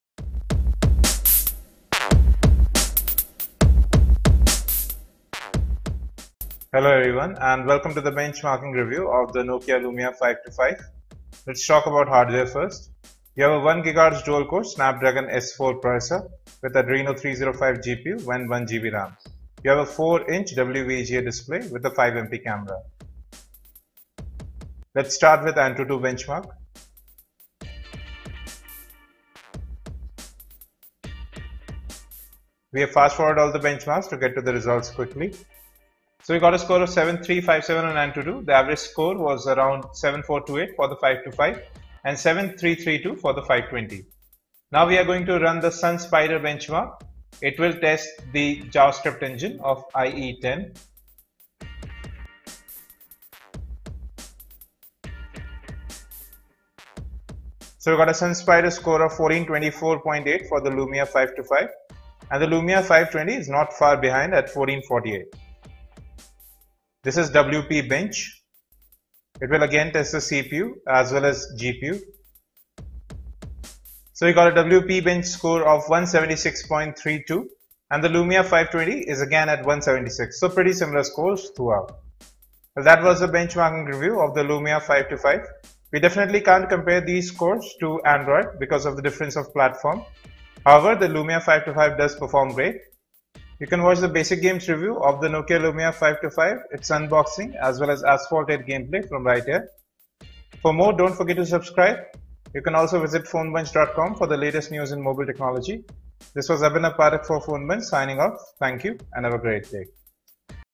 Nokia Lumia 525 Benchmarks
Benchmarks of the Nokia Lumia 525 and score comparison with the Lumia 520.